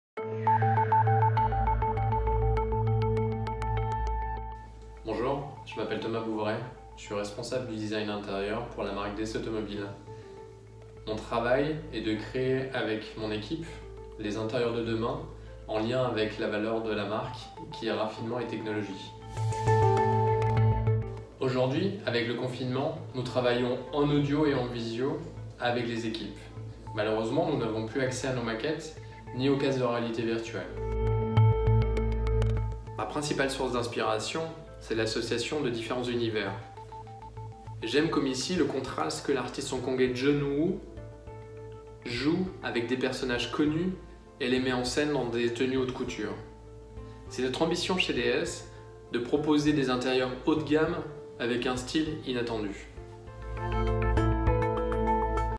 DS EXPERTS - TROIS QUESTIONS À THOMAS BOUVERET
Comment créer l'équilibre parfait entre sophistication et technologie ? Thomas Bouveret, notre responsable Design Intérieur, nous parle de ses inspirations et de son savoir-faire dans ce nouvel épisode de DS Experts.